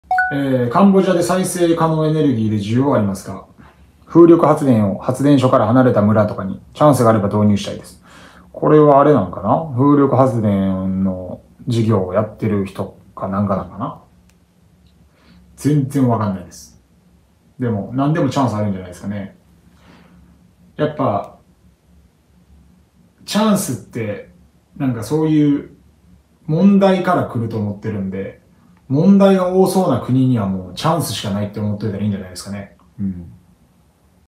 分からないことを隠さずに言う正直なケイスケホンダ【本田圭佑切り抜き】
本田圭佑選手の切り抜きをしています！
公認目指して1日5本投稿してます！
元動画！

ご本家様のYouTubeはこちらから！

本田圭佑（ほんだ けいすけ、1986年（昭和61年）6月13日 - ）は、大阪府摂津市出身のプロサッカー選手。ポジションはミッドフィールダー。元日本代表。サッカー指導者、サッカークラブ経営者、実業家。カンボジア代表のGM・監督を務める傍ら慈善活動にも取り組む。　引用-Wikipedia